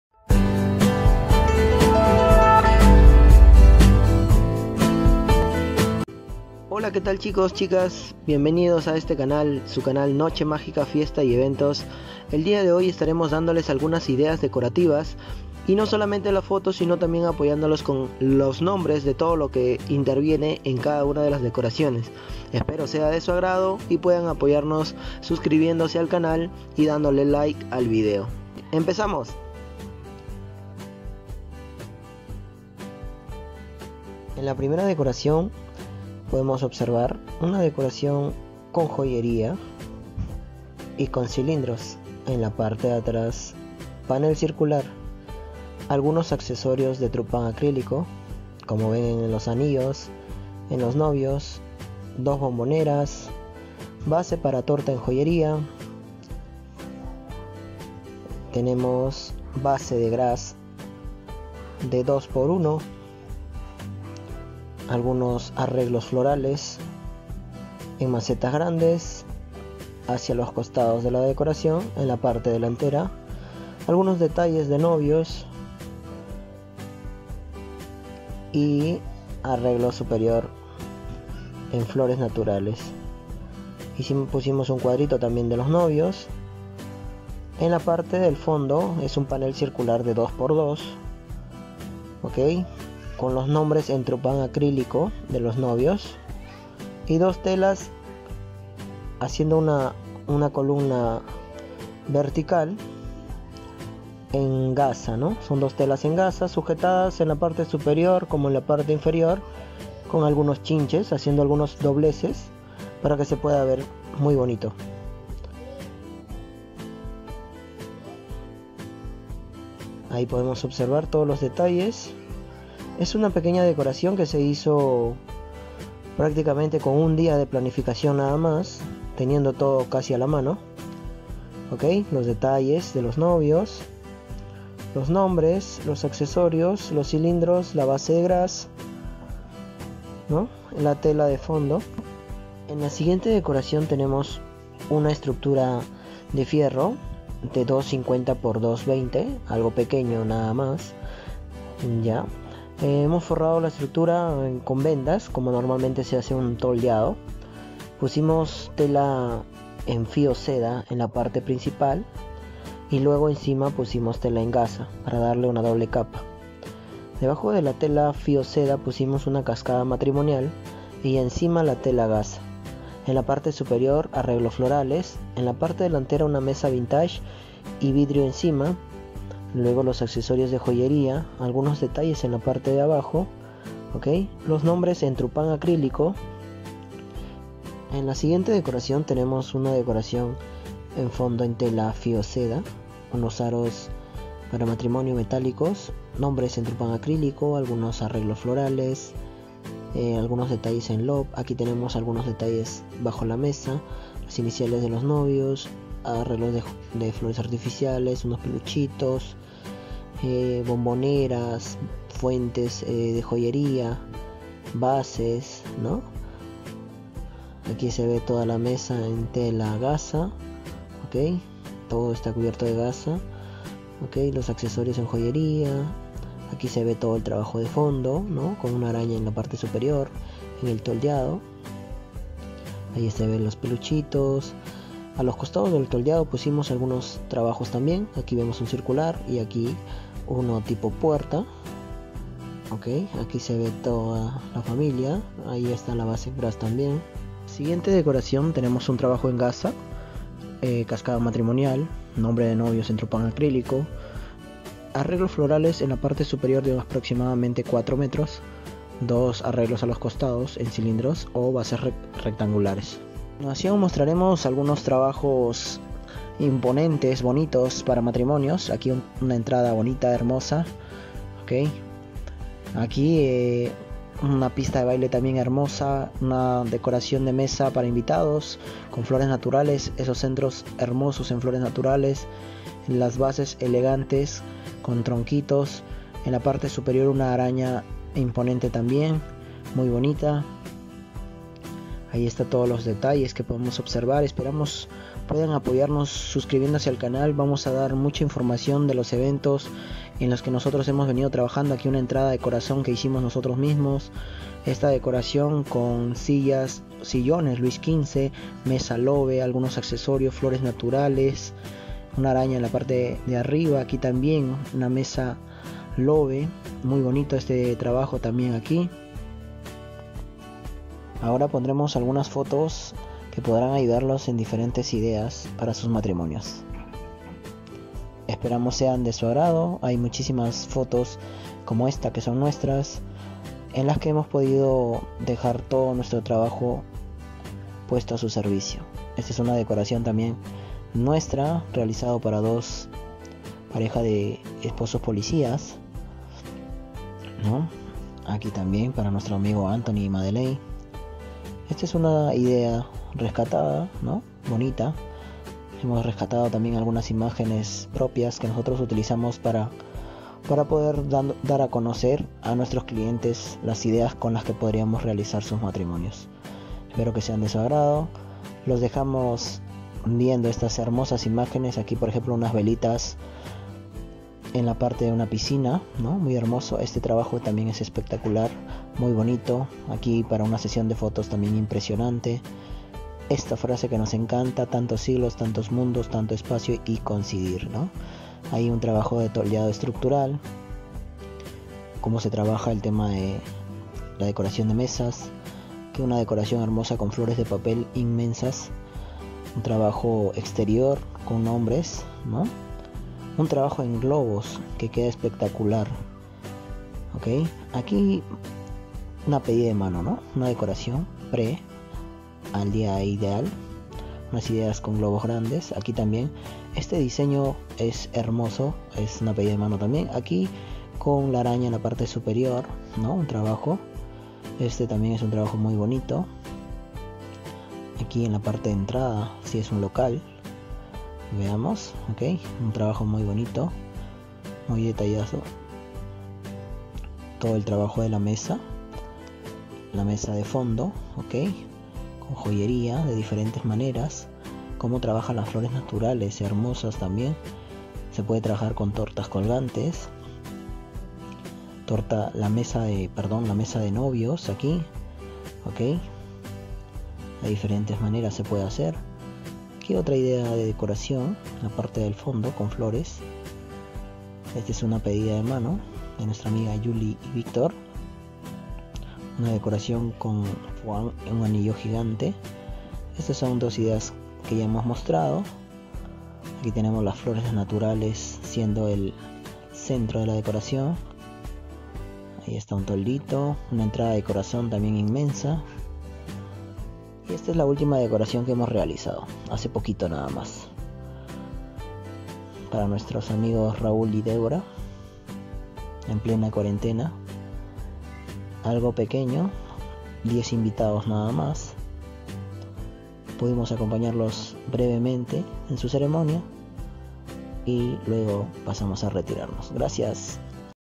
30 Ideas de decoraciones para Matrimonio TENDENCIAS
A continuación algunas ideas fabulosas para su boda. Ideas brevemente explicaras si eres un decorador y quieres tener algunas ideas.